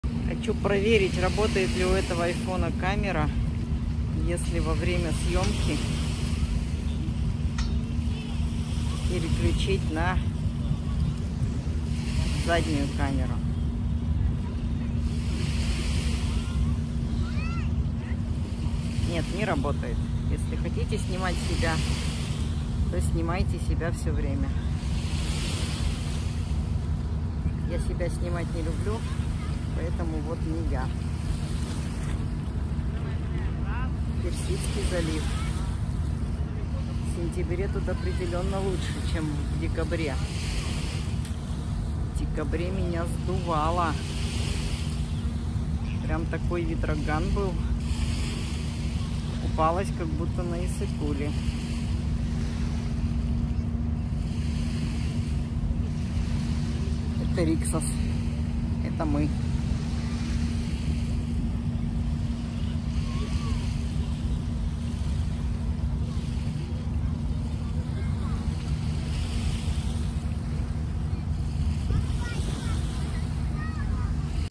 Пляж Movenpick Hotel Jumeirah Beach 5*. Dubai. UAE
Отдых за пределами Казахстана во время пандемии Covid 19